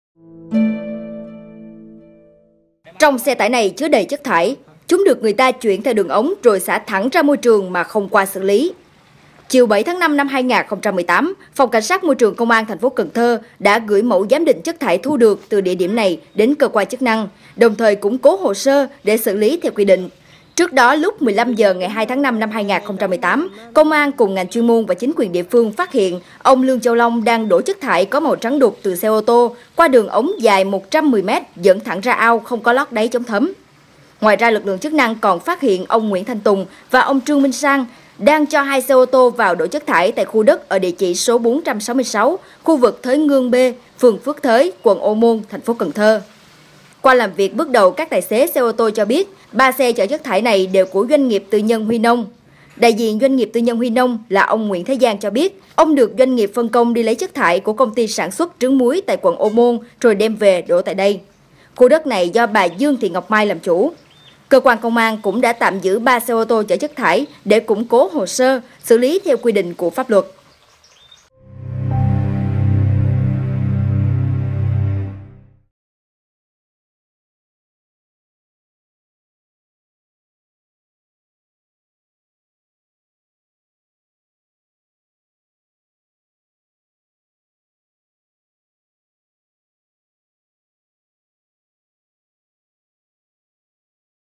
Kinh hãi cảnh xả chất thải màu trắng đục thẳng ra môi trường
Chiều 7.5.2018, Phòng Cảnh sát Môi trường Công an TP.Cần Thơ đã gửi mẫu giám định chất thải thu được từ địa điểm này đến cơ quan chức năng, đồng thời củng cố hồ sơ để xử lý theo quy định.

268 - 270 Nguyễn Đình Chiểu, Phường 6, Q.3, TP HCM